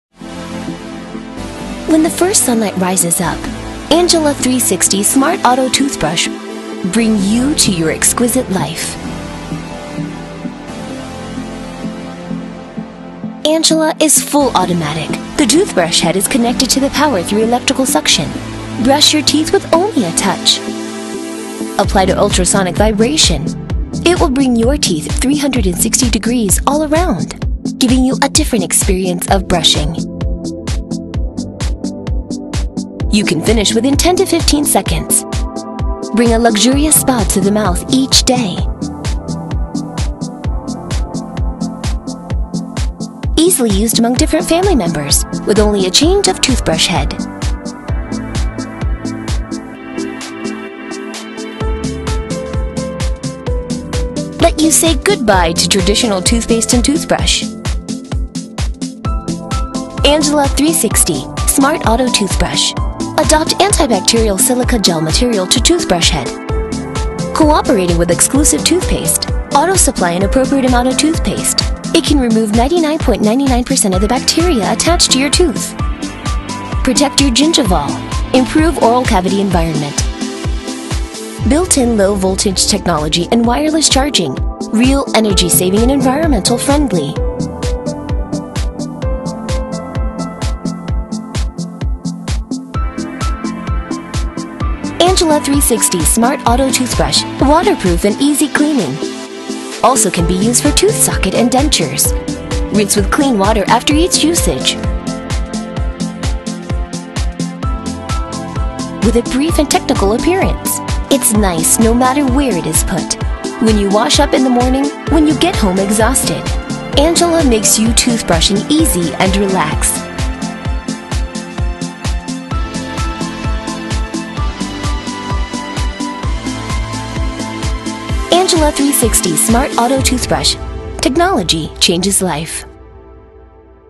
ANJIELA Automatic Electric Wireless Charging Toothbrush White
Features:
Unique U-shape Bite Design: U-shaped bite is designed to fit the mouth shape, can clean the surface and interior of the teeth in all directions. Made by food grade antibacterial silica gel, super waterproof, can be cleaned and reused, can kill 99.99% of the bacteria in the mouth.
360 Degree Brushing: By ultrasonic technology, delivering enormous energy to the toothbrush head, combined with U type toothbrush top innovation function and unique design, using only a 10-15 seconds to complete a 360 degree omnibearing brushing.
Magnetic Connection: A seamless connection allows the toothbrush head to be closely connected to the head, without worrying about the problem of the power head shedding due to vibration when you brush your teeth.
Touch Switch: A touch switch at the bottom of the toothbrush, discard the excess control button, no longer need to worry about the complicated operation, truly experience the delicate life.
Emancipate Your Hands: U type single mouthpiece design, so you brush your teeth to liberate your hands, improve the efficiency of your life, save your valuable time.
Wireless Charging: 10 minutes a charge, can be used for more than 20 days, truly energy saving and environmental protection.
2 Toothbrush Heads: With 2 brush heads inside, convenient for home use, durable for a long time use.